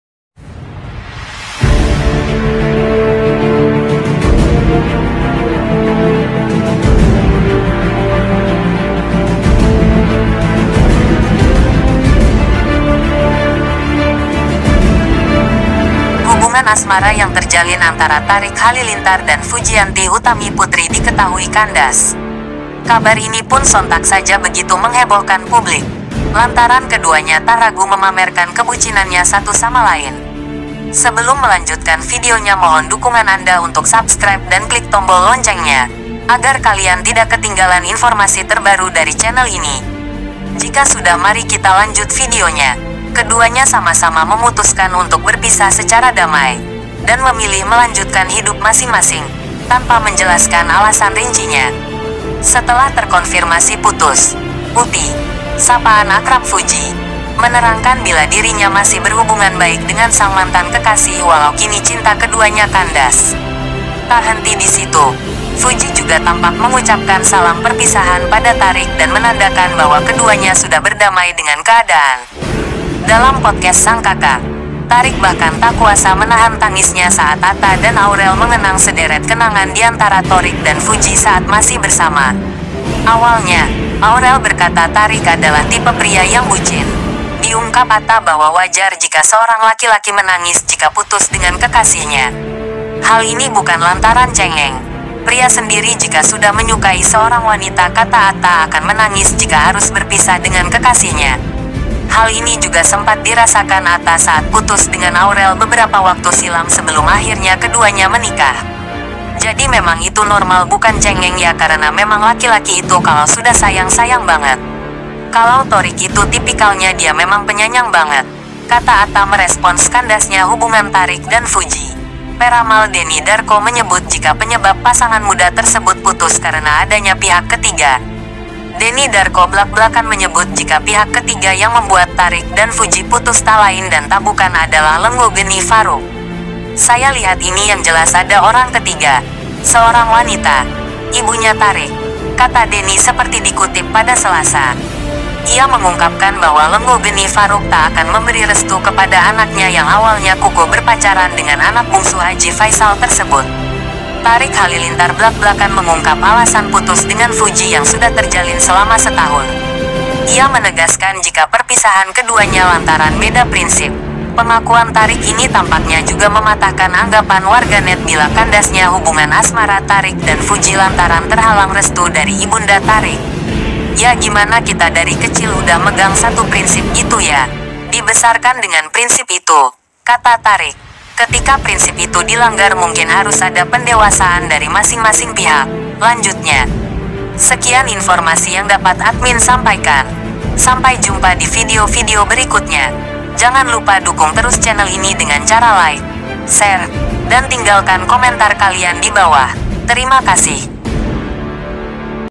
MENGEJUTKAN !!! RAMALAN DENNY DARKO, INI PENYEBAB THORIQ FUJI BISA BERSATU KEMBALI
MENGEJUTKAN ramalan Deny Darko tentang hubungan Thoriq dan fuji

Hubungan asmara yang terjalin antara Thariq Halilintar dan Fujianti Utami Putri diketahui kandas. Kabar ini pun sontak saja begitu menghebohkan publik, lantaran keduanya tak ragu memamerkan kebucinannya satu sama lain.

Keduanya sama-sama memutuskan untuk berpisah secara damai, dan memilih melanjutkan hidup masing-masing, tanpa menjelaskan alasan rincinya.
Setelah terkonfirmasi putus, Uti, sapaan akrab Fuji, menerangkan bila dirinya masih berhubungan baik dengan sang mantan kekasih walau kini cinta keduanya kandas.
Tak Henti di situ, Fuji juga tampak mengucapkan salam perpisahan pada Thariq dan menandakan bahwa keduanya sudah berdamai dengan keadaan.

Dalam podcast sang kakak , Thariq bahkan tak kuasa menahan tangisnya saat Atta dan Aurel mengenang sederet kenangan diantara Thoriq dan Fuji saat masih bersama. Awalnya, Aurel berkata Thariq adalah tipe pria yang bucin. Diungkap Atta bahwa wajar jika seorang laki-laki menangis jika putus dengan kekasihnya. Hal ini bukan lantaran cengeng, pria sendiri jika sudah menyukai seorang wanita kata Atta akan menangis jika harus berpisah dengan kekasihnya. Hal ini juga sempat dirasakan Atta saat putus dengan Aurel beberapa waktu silam sebelum akhirnya keduanya menikah. jadi memang itu normal bukan cengeng ya karena memang laki-laki itu kalau sudah sayang sayang banget. Kalau Thoriq itu tipikalnya dia memang penyayang banget," kata

Merespons kandasnya hubungan Thariq dan Fuji, peramal Denny Darko menyebut jika penyebab pasangan muda tersebut putus karena adanya pihak ketiga. Denny Darko blak-blakan menyebut jika pihak ketiga yang membuat Thariq dan Fuji putus tak lain dan tak bukan adalah Lenggogeni Faruk.
"Saya lihat ini yang jelas ada orang ketiga, seorang wanita, ibunya Thariq," kata Denny seperti dikutip pada Selasa
Ia mengungkapkan bahwa Lenggogeni Faruk tak akan memberi restu kepada anaknya yang awalnya kukuh berpacaran dengan anak bungsu Haji Faisal tersebut.

Thariq Halilintar blak-blakan mengungkap alasan putus dengan Fuji yang sudah terjalin selama setahun. Ia menegaskan jika perpisahan keduanya lantaran beda prinsip.
Pengakuan Thariq ini tampaknya juga mematahkan anggapan warganet bila kandasnya hubungan asmara Thariq dan Fuji lantaran terhalang restu dari ibunda Thariq.

Ya gimana kita dari kecil udah megang satu prinsip gitu ya. Dibesarkan dengan prinsip itu," kata Thariq.
"Ketika prinsip itu dilanggar mungkin harus ada pendewasaan dari masing-masing pihak," lanjutnya.